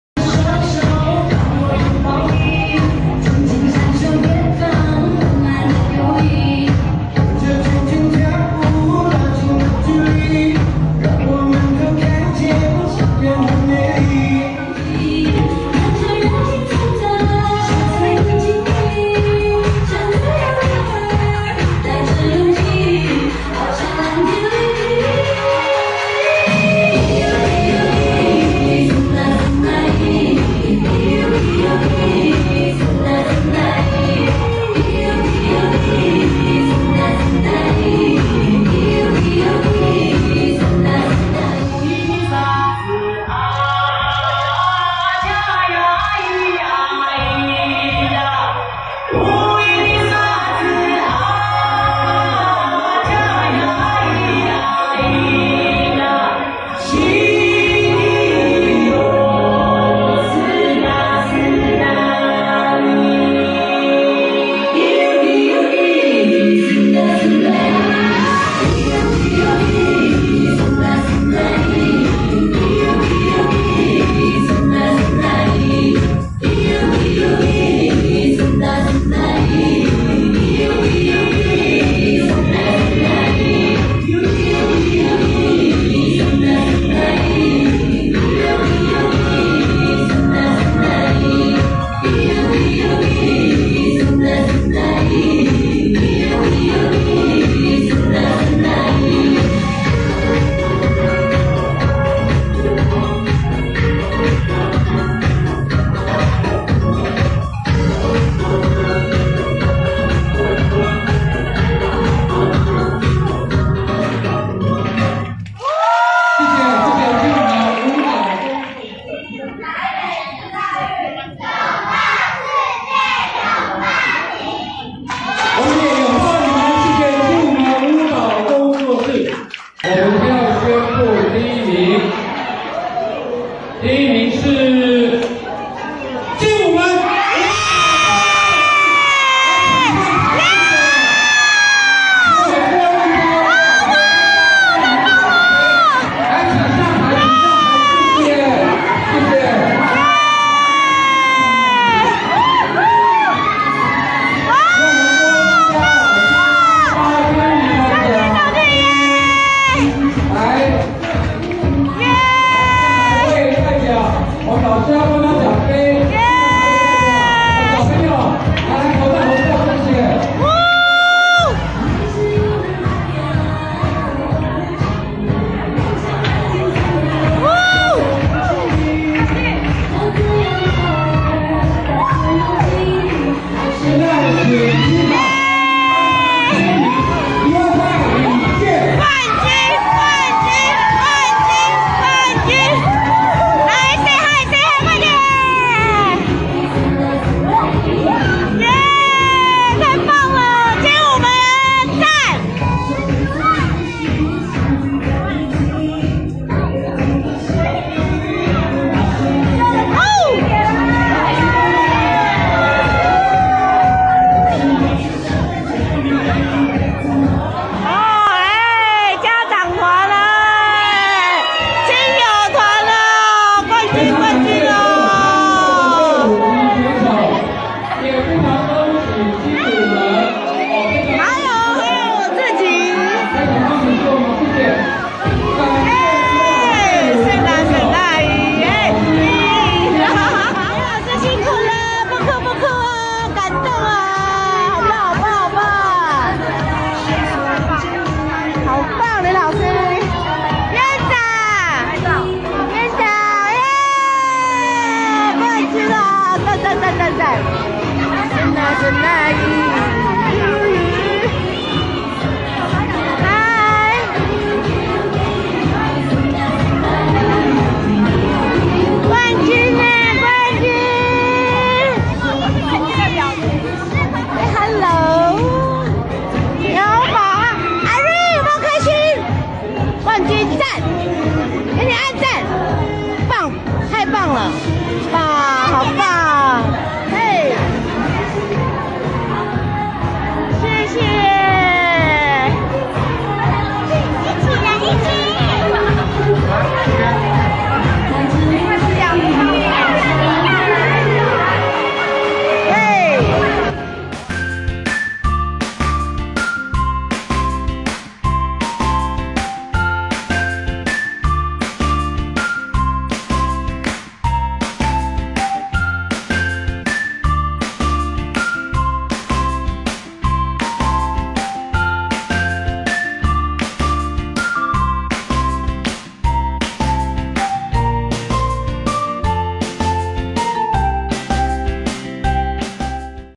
Katy'life/京舞門520台北世大運全記錄
2017/5/20京舞門舞蹈工作室
台北世大運競舞比賽全紀錄（比賽片段、花絮）